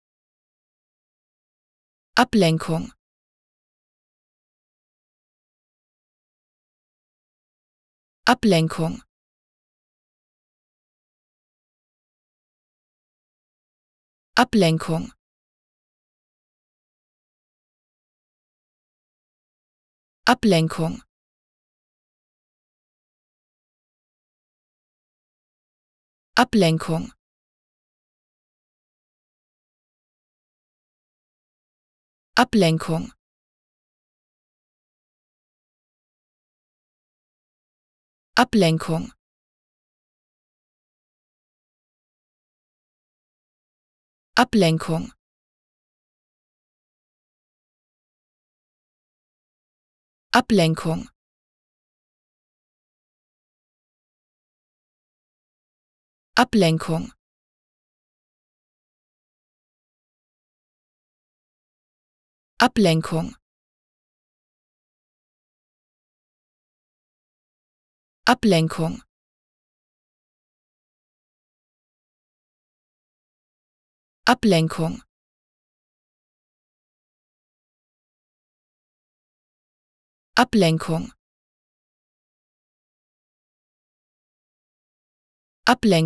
How to Pronounce ''Ablenkung'' (Distraction) Correctly in German
Learn how to say ''Ablenkung'' in German with this short pronunciation tutorial.